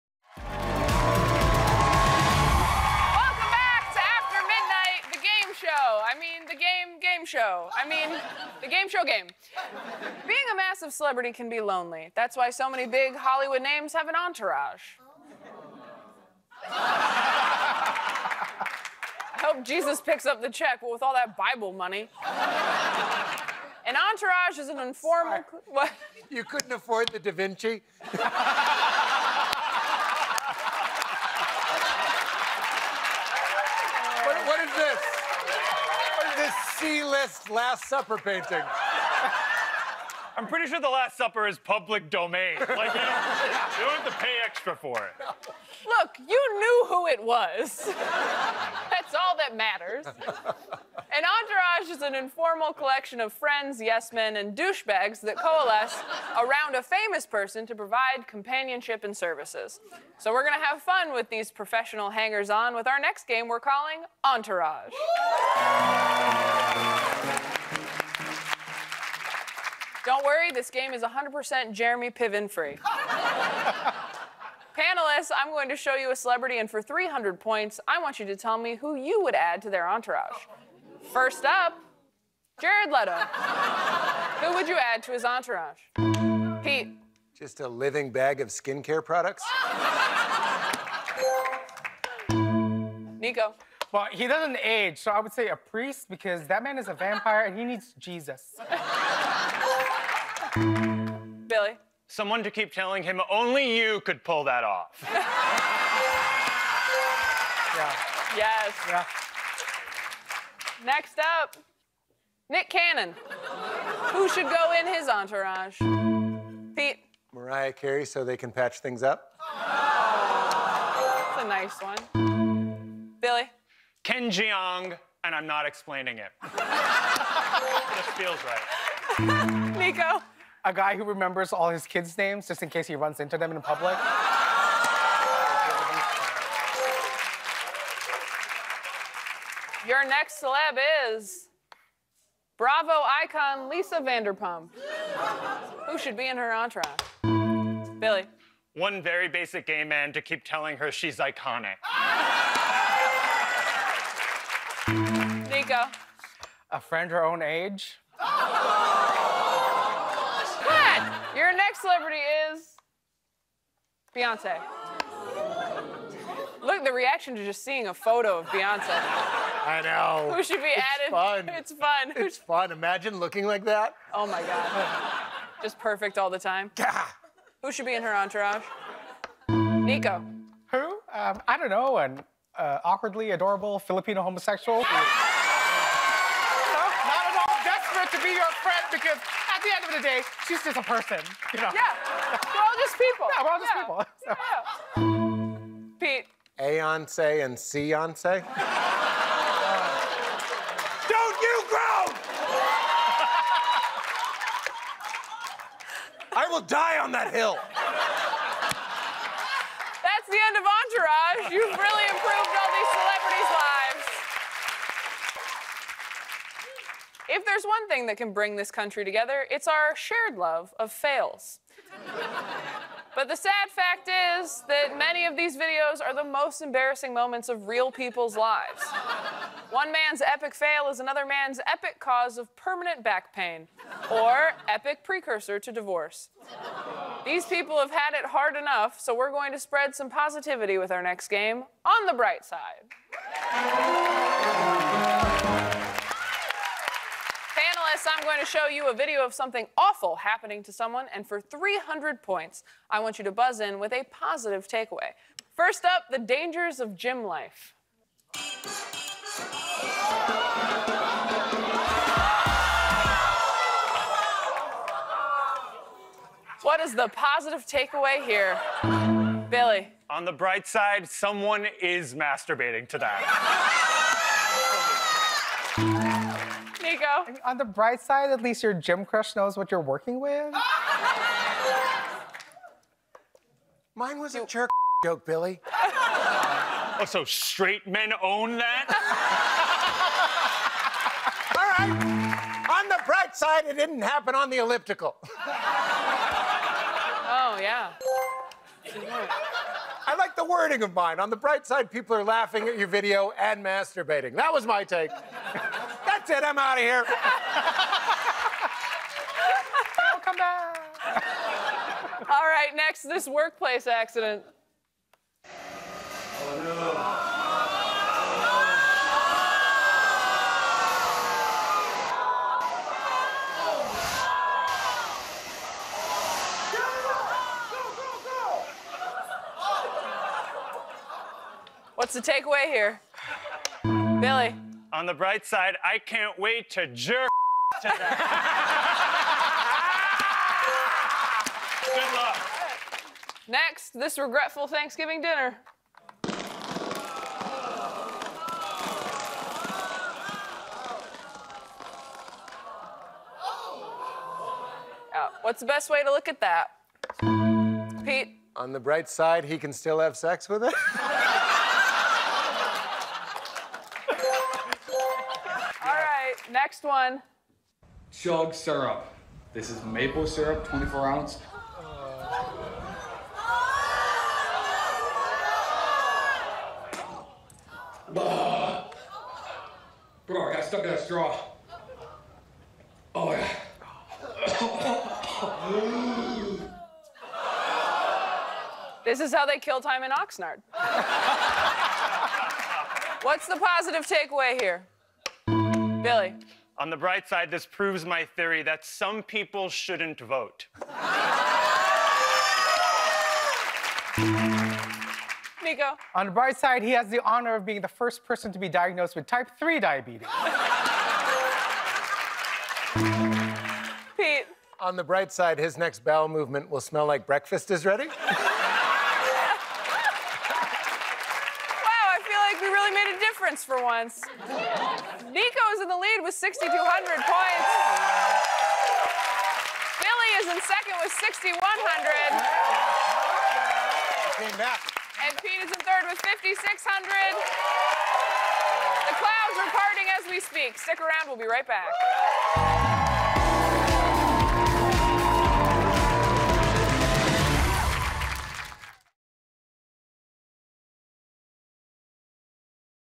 Pete Holmes Finds the Silver Lining in Multiple Viral Catastrophes
Taylor Tomlinson asks Nico Santos, Billy Eichner, and Pete Holmes who celebrities like Beyonce and Lisa Vanderpump should add to their entourage. Later, panelists do their best to look on the bright side of bleak viral videos.